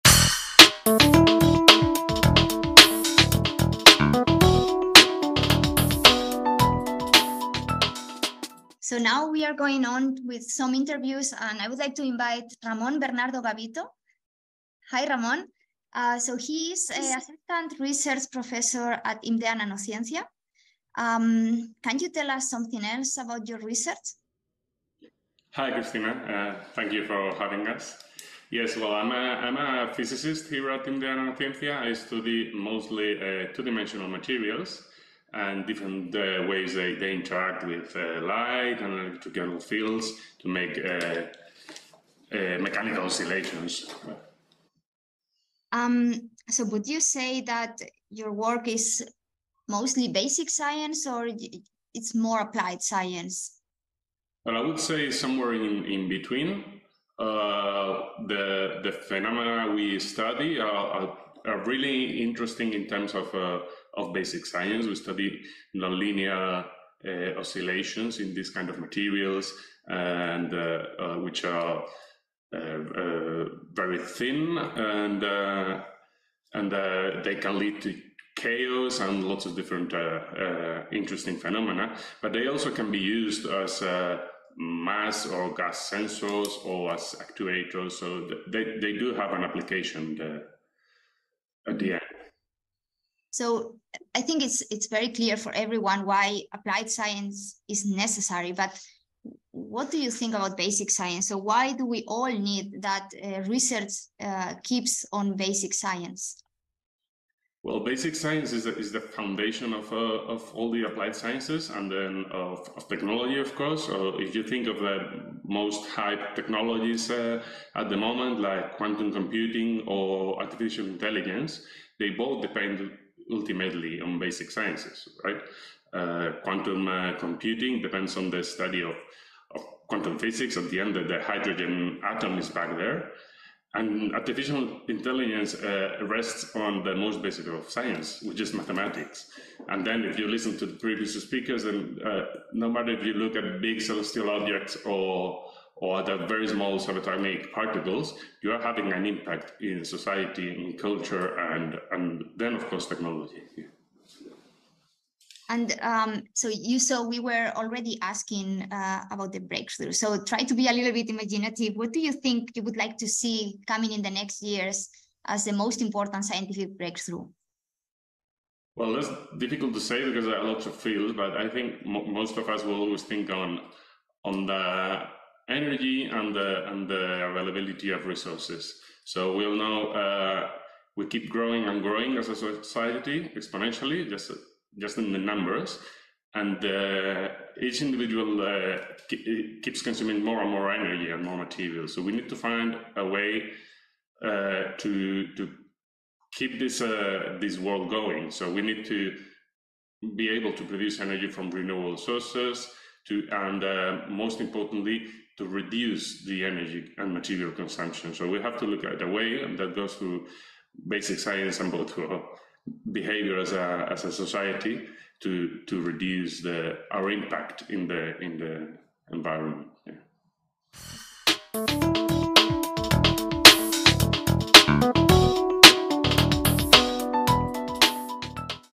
Conversation with Younger Scientists - Ramon Bernardo Gavito
Now we are going on with some interviews. And I would like to invite Ramon Bernardo Gavito.
He is assistant research professor at Imdea Nanociencia. Can you tell us something else about your research?

Hi, Christina. Thank you for having us. I'm a physicist here at Imdea Nanociencia. I study mostly two dimensional materials and different ways they interact with light and electromagnetic fields to make mechanical oscillations.

So would you say that your work is mostly basic science, or it's more applied science?

I would say somewhere in between. The phenomena we study are really interesting in terms of basic science. We study non-linear oscillations in these kind of materials, which are very thin, and they can lead to chaos and lots of different interesting phenomena. But they also can be used as mass or gas sensors or as actuators. So they do have an application, at the end. So I think it's very clear for everyone why applied science is necessary.

But what do you think about basic science? Why do we all need that research keeps on basic science?

Basic science is the foundation of all the applied sciences and then of technology, of course. If you think of the most high technologies at the moment, like quantum computing or artificial intelligence, they both depend ultimately on basic sciences. Quantum computing depends on the study of quantum physics at the end, the hydrogen atom is back there. And artificial intelligence rests on the most basic of science, which is mathematics. And then if you listen to the previous speakers, and no matter if you look at big cell, still objects or the very small accelerator-made particles, you are having an impact in society, in culture and then, of course, technology.

And you said we were already asking about the breakthrough. So try to be a little bit imaginative. What do you think you would like to see coming in the next years as the most important scientific breakthrough?

Well, it's difficult to say because I have a lot of fields, but I think most of us will always think on the energy and the availability of resources. We keep growing and growing as a society, exponentially, just in the numbers. And each individual keeps consuming more and more energy and more materials. So we need to find a way to keep this world going. We need to be able to produce energy from renewable sources and, most importantly, to reduce the energy and material consumption. We have to look at the way, and that goes through basic science and both to behavior as a society, to reduce our impact in the environment.